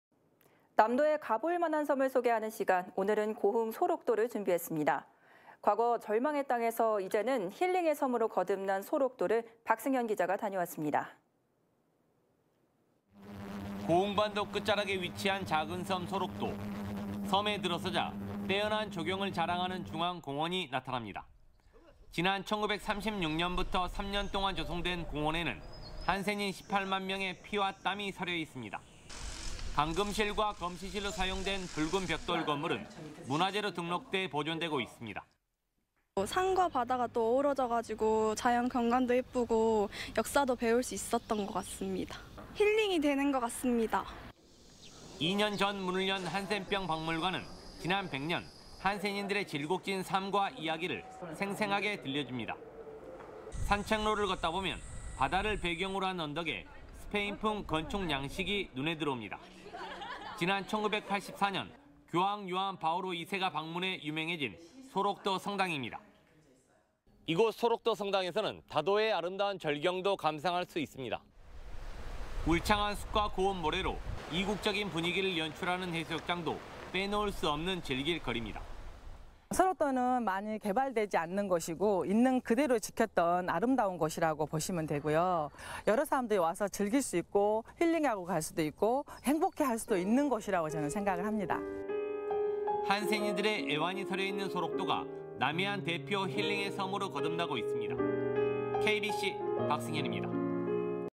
[섬으로 떠나자!] 힐링의 섬, 고흥 소록도
남도의 가볼만한 섬을 소개하는 시간, 오늘은 고흥 소록도를 준비했습니다.

과거 절망의 땅에서 이제는 힐링의 섬으로 거듭난 소록도를 박승현 기자가 다녀왔습니다.